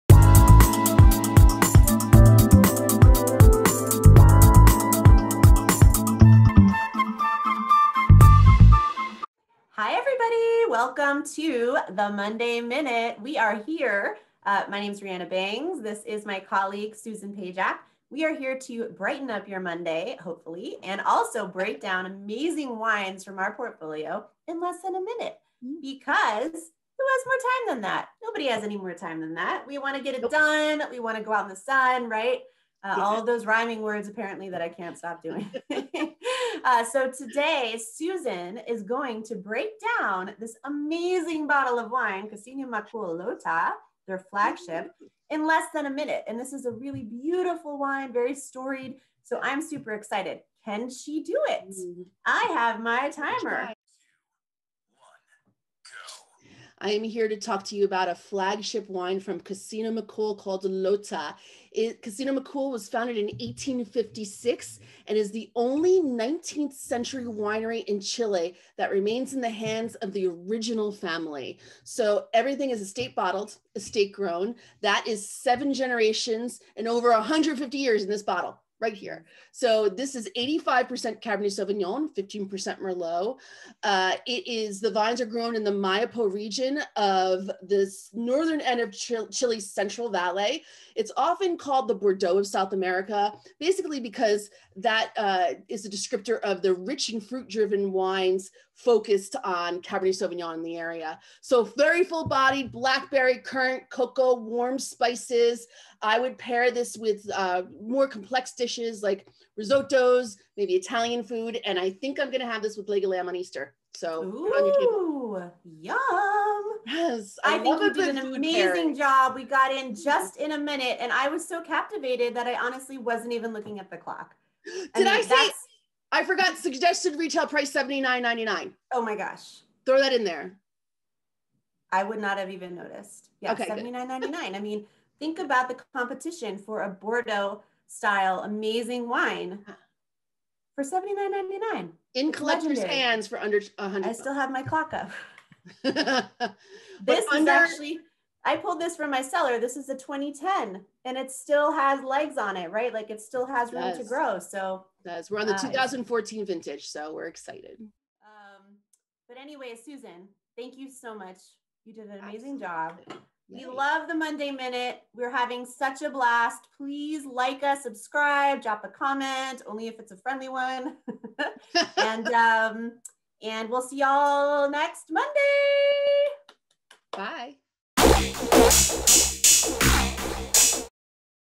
The Monday Minute - Presenting Cousino Macul Lota
Challenge; Create and Execute an impactful wine presentation in under one minute. Can we do it? Check out how Rhianna Bangs and Susan Pajak present the features and benefits focused on Cousino Macul "Lota" - you be the judge!
Cousino Macul, established in 1856, is the only 19th century winery, in Chile, that remains in the hands of the original family. That is seven generations, and over 150 years in the bottle!
"Lota" is one of the flagship wines from the winery - 85% Cabernet Sauvignon, 15% Merlot. The vines chosen for this blend are located in the Maipo region of Chile's Central Valley. This area is often called "The Bordeaux of South America" because of the rich and fruit driven wines focused on Cabernet Sauvignon. These vines are planted at the foot of the Andes Mountain range; 600 feet above sea level.. This elevation supports in the ripeness and acidity of the grapes as well as in the balance the wine displays.
Flavors of dark fruit and cedar lead your palate into a full bodied wine with a finish of cocoa and warm spices. Pair this wine with complex dishes like risottos, Italian inspired entrees, and heartier meats such as beef and lamb.